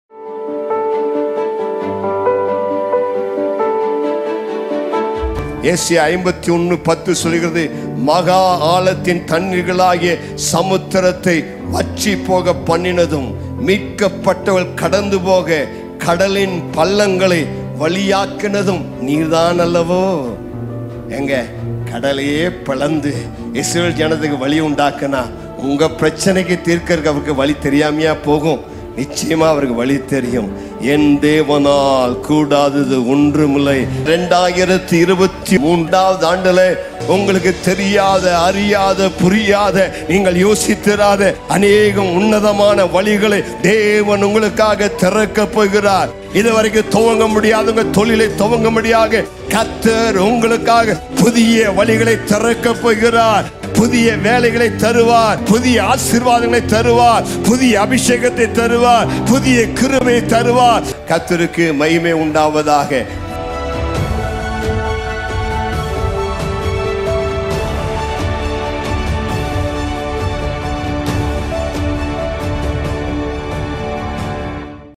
புதிய கிருபை | சங்கை டேவிட் பிரகாசம்
Message By Rev.Dr.David Prakasam
@Bethel City Cathedral
4C-BKR Nagar, Sathy Road,
Coimbatore - 641012 India.